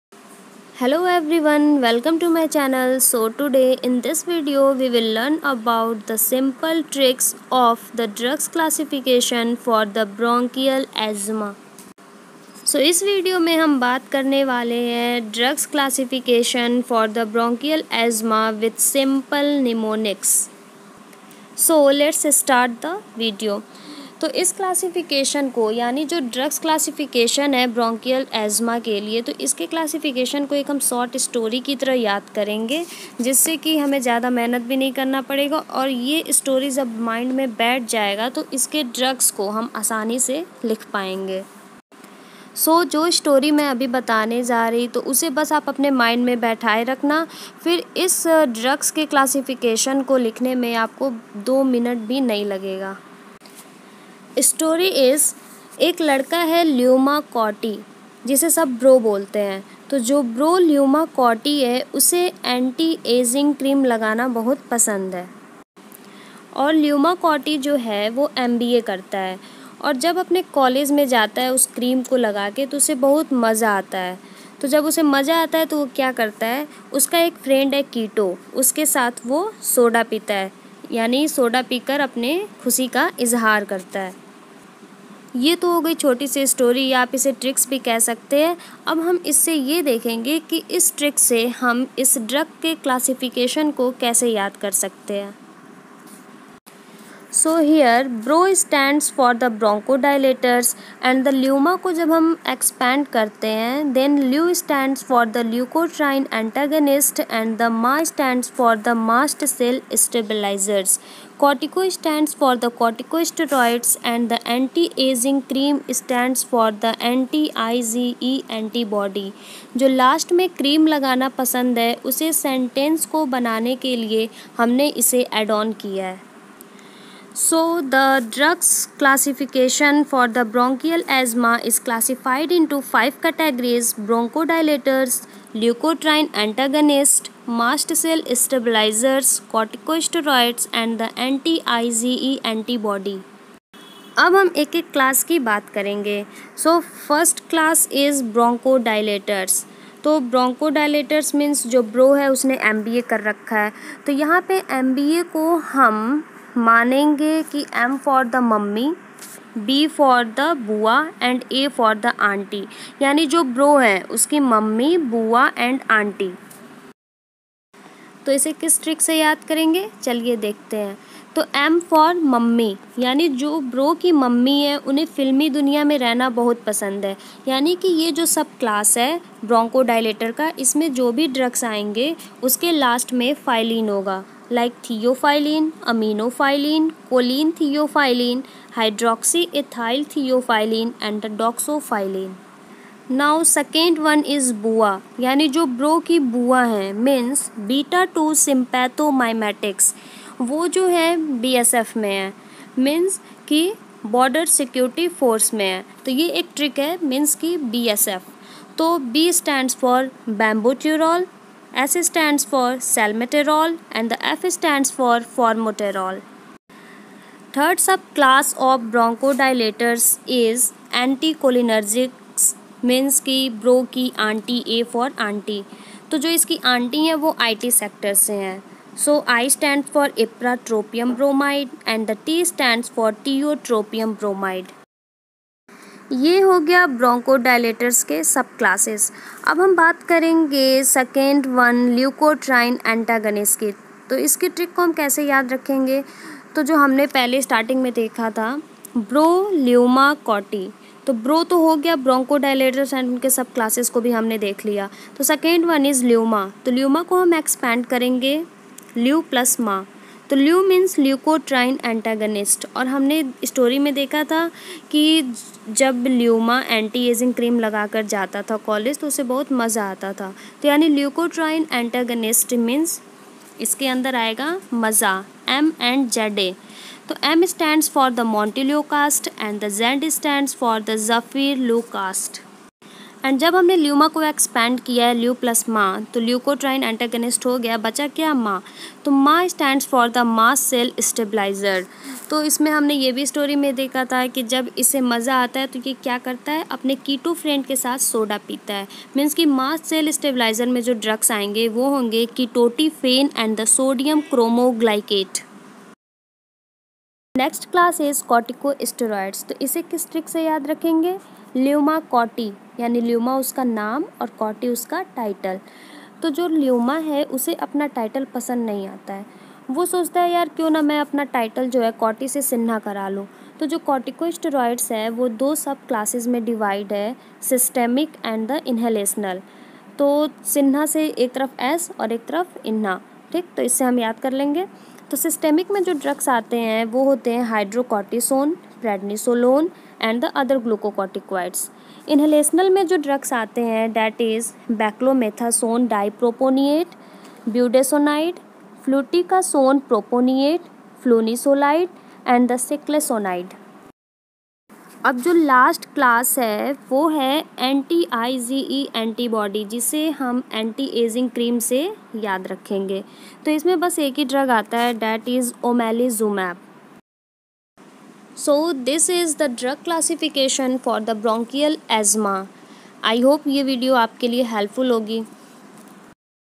CLASSIFICATION || DRUGS FOR BRONCHIAL ASTHMA || VERY EASY TRICKS TO LEARN
In this video we will learn the classification of drugs for bronchial asthma using simple and easy tricks.